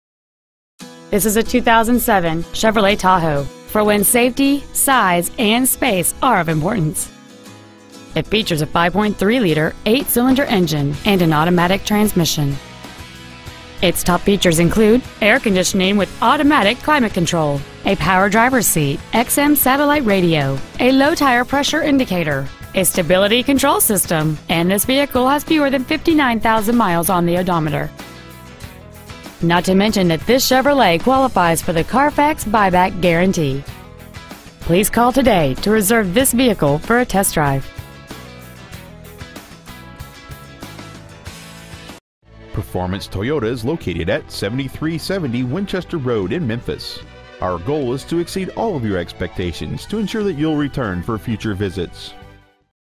Pre-Owned 2007 Chevrolet Tahoe Memphis TN
Year: 2007
Make: Chevrolet
Model: Tahoe
Engine: VORTEC 5.3L V8 SFI
Trans.: Automatic
Exterior: Black
Miles: 58,480

Performance Toyota
7370 Winchester Road